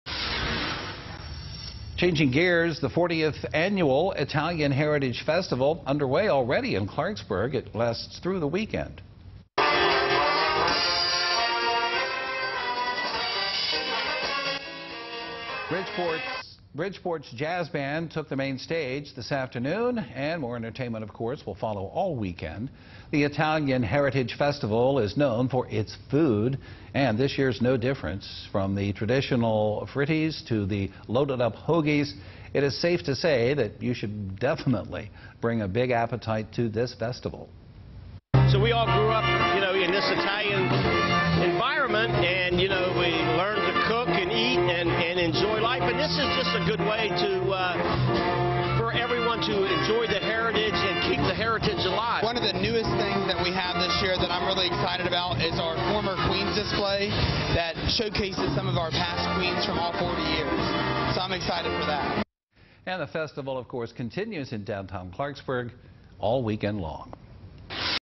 40th Annual Italian Heritage Festival brings sounds and smells of Italy to downtown Clarksburg
40th Annual Italian Heritage Festival brings sounds and smells of Italy to downtown Clarksburg